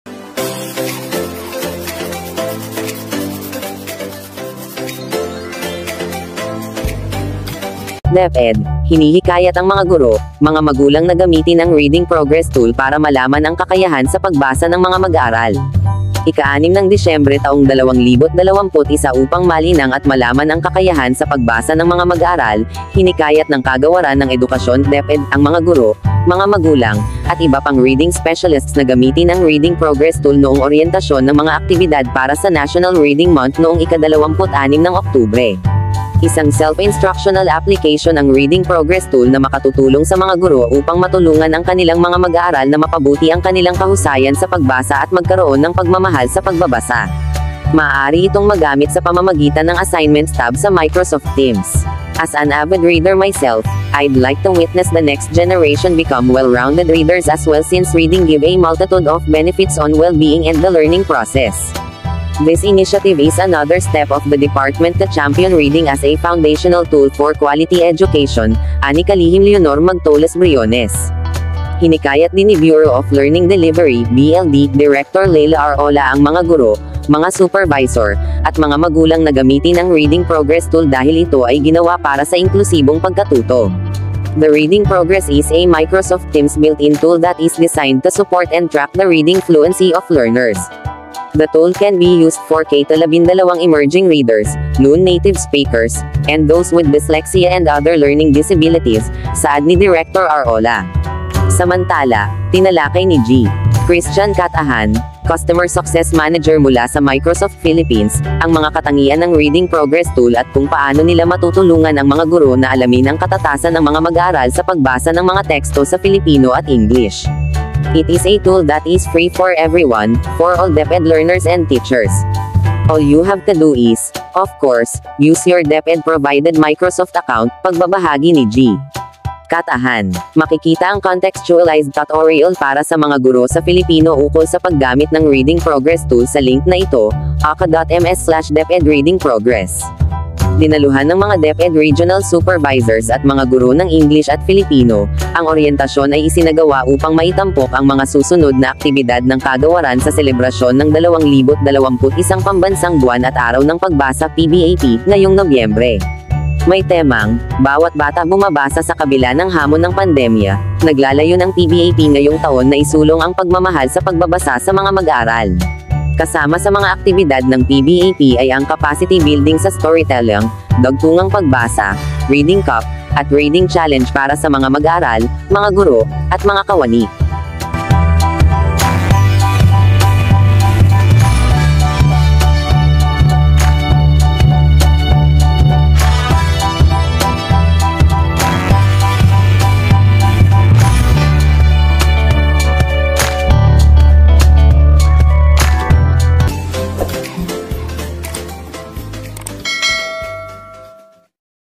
DEPED, HINIHIKAYAT ANG MGA GURO, MGA MAGULANG NA GAMITIN ANG READING PROGRESS TOOL PARA MALAMAN....
Video Title: DEPED, HINIHIKAYAT ANG MGA GURO, MGA MAGULANG NA GAMITIN ANG READING PROGRESS TOOL PARA MALAMAN ANG KAKAYAHAN SA PAGBASA NG MGA MAG-AARAL

Kung inyo pong nagustuhan, huwag natin kalimutang magLIKE, iSHARE at magSUBSCRIBE sa channel na ito.

Maraming salamat kaGuro!

God bless us all kaGuro!